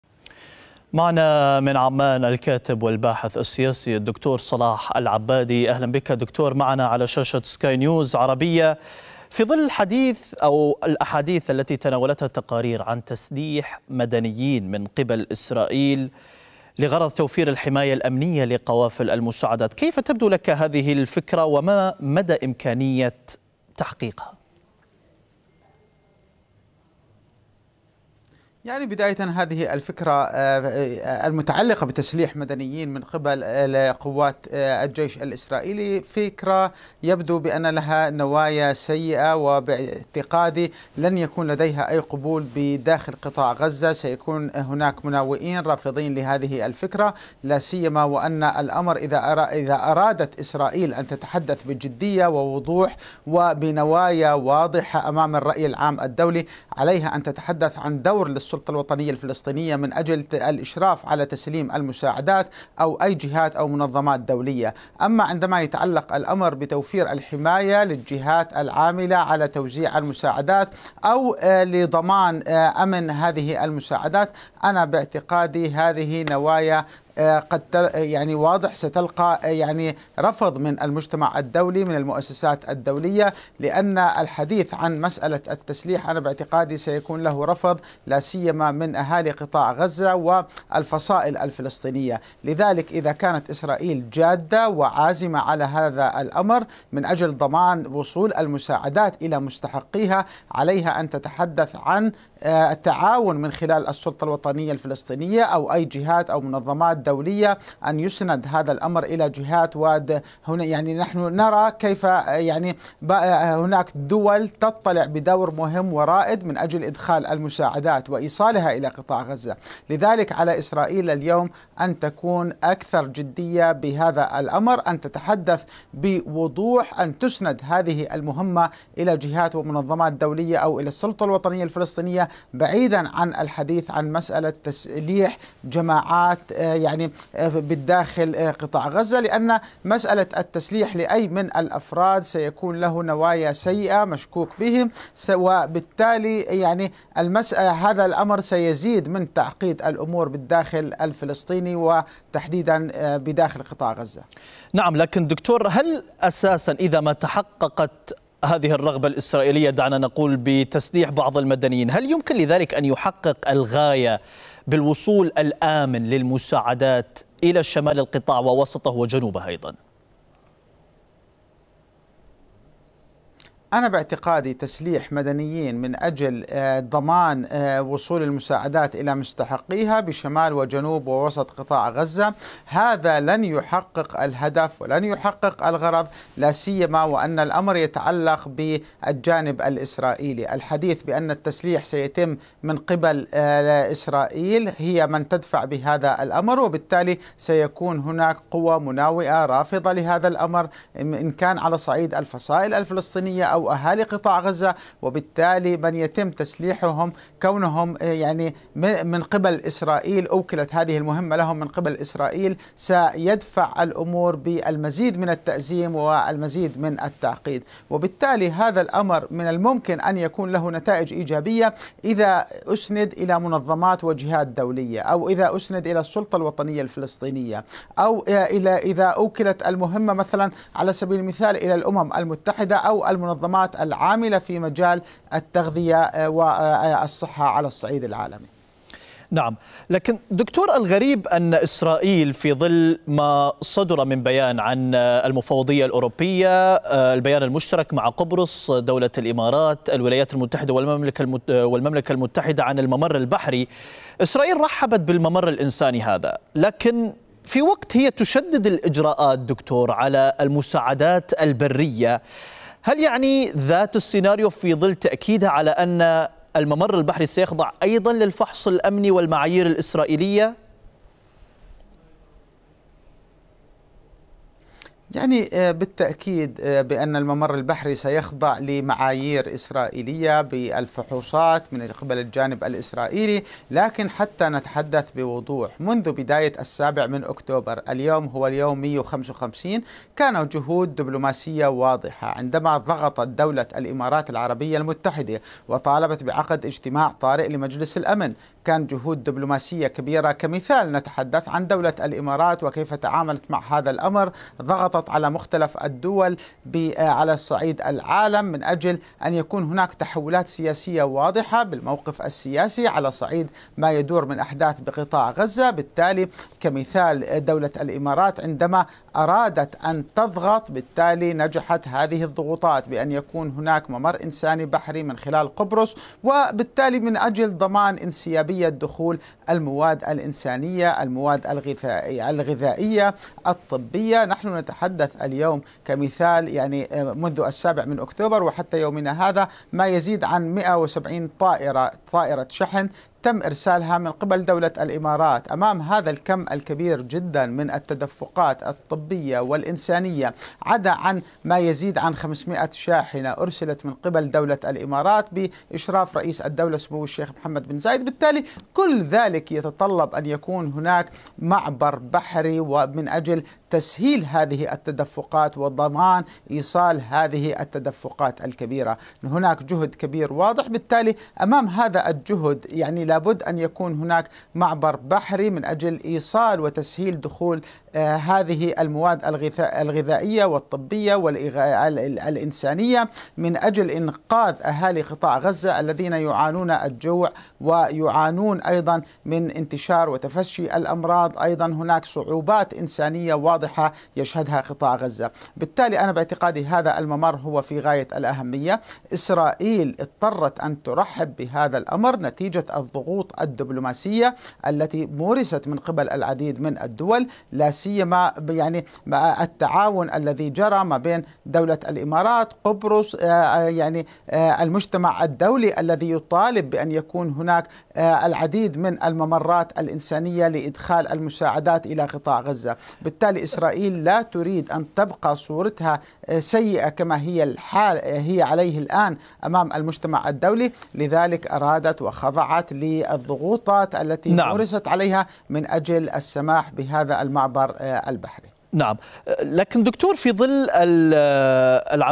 ما مدى إمكانية تسليح مدنيين في غزة لحماية المساعدات؟
قالت صحيفة (إسرائيل هيوم) إن مسؤولين إسرائيليين يناقشون تسليح بعض المدنيين في قطاع غزة لتوفير الحماية الأمنية لقوافل المساعدات المتجهة إلى القطاع.